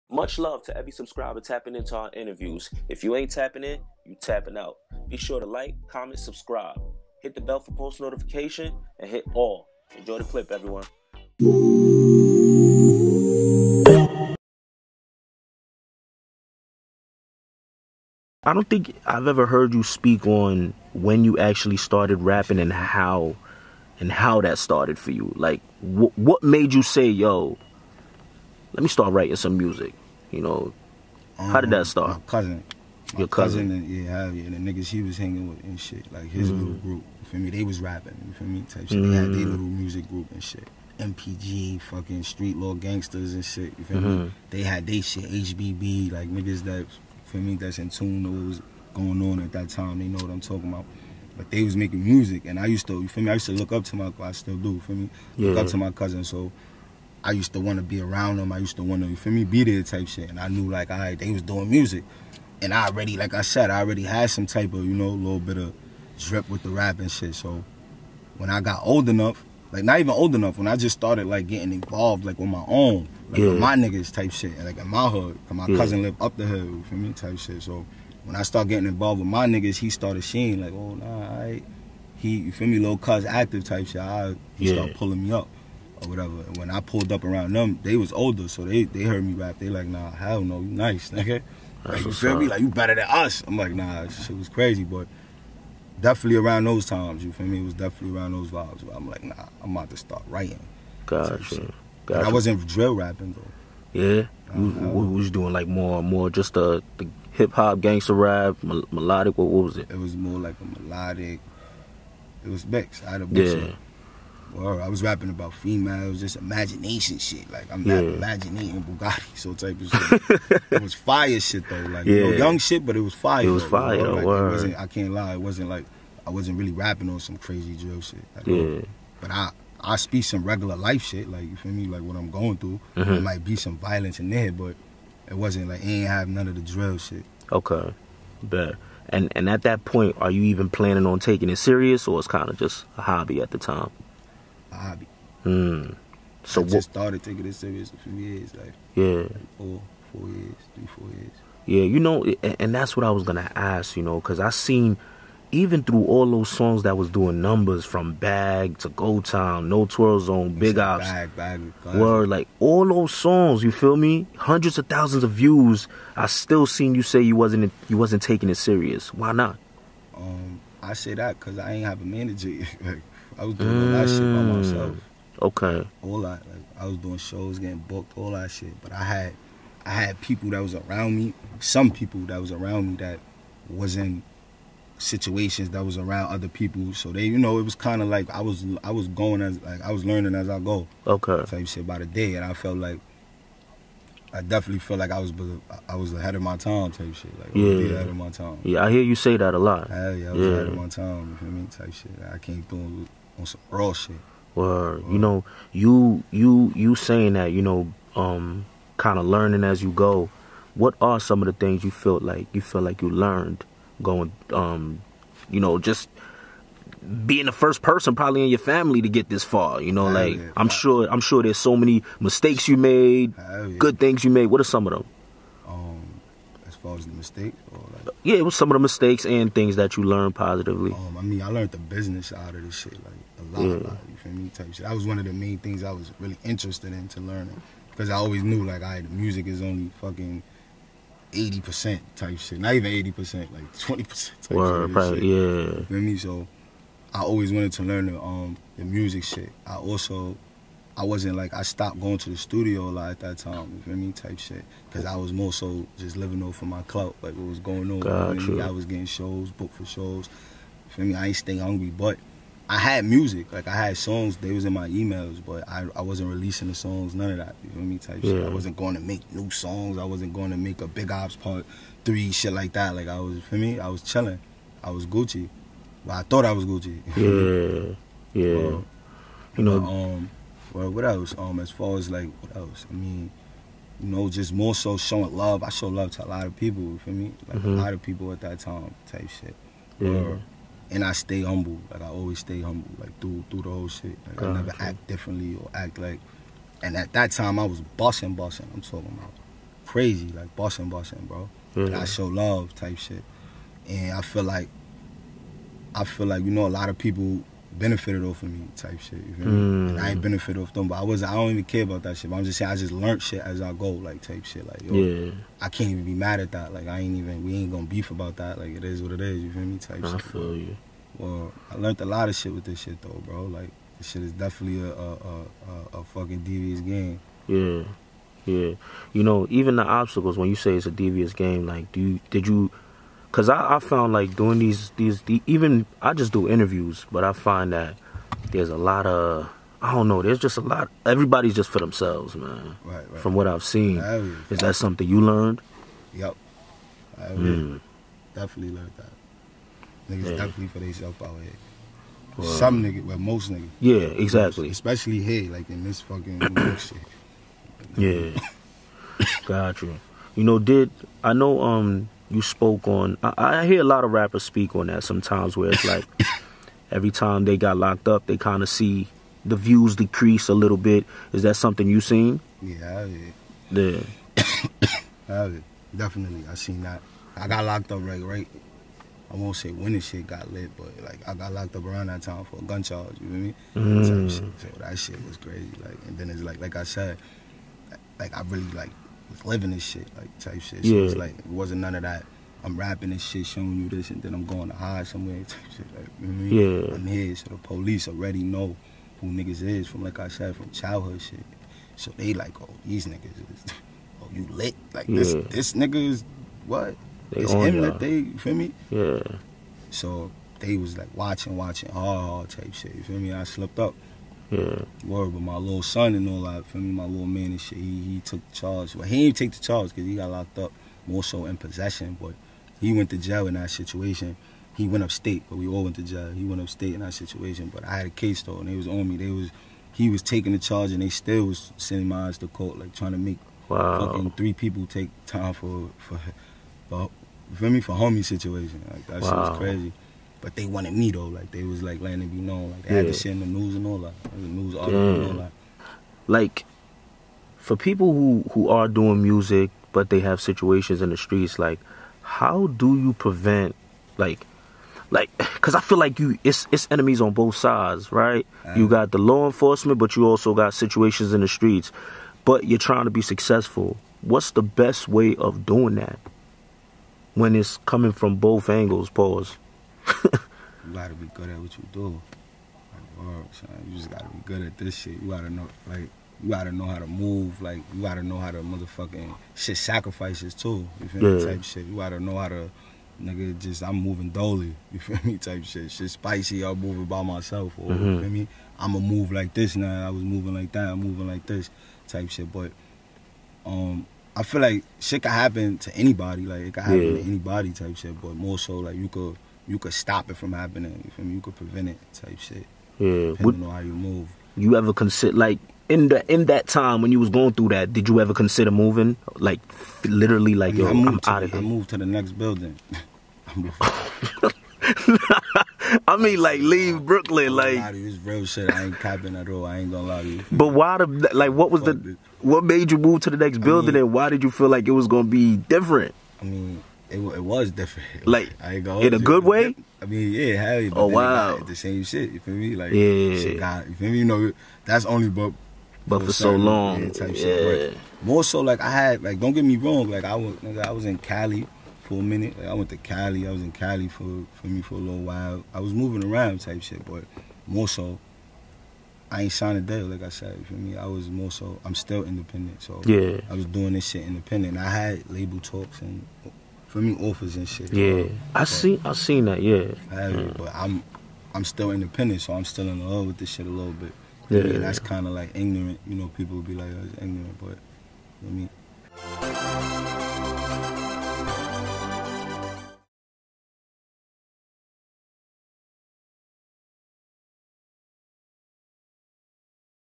Jezz Gasoline On How He Started Rapping | Seeing Views Decrease Every Time He Got Locked Up (Part 7)
In this clip, Jezz Gasoline speaks on how he began rapping and taking it serious.

PPhilms stays tapped in with your favorite artists and creatives.